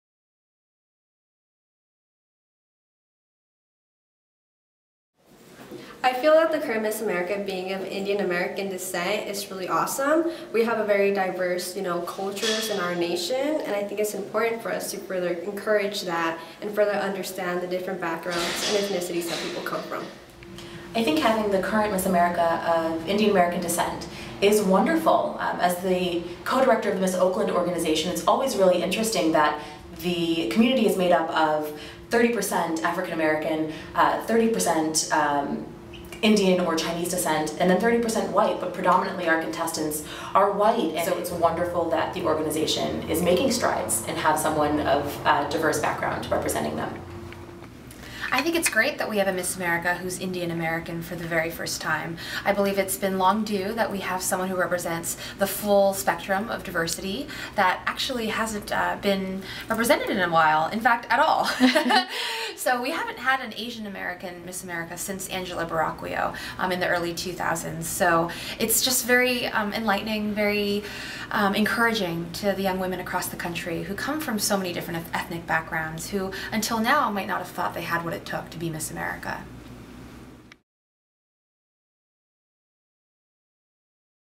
Diversity in the Miss America Organization
For my MCOM 63 (New Media) our very assignment was to create a video project on interviewing to three people about a current event question. Hope you enjoy! I know my Miss CAOrg pageant friends will get a kick out of this.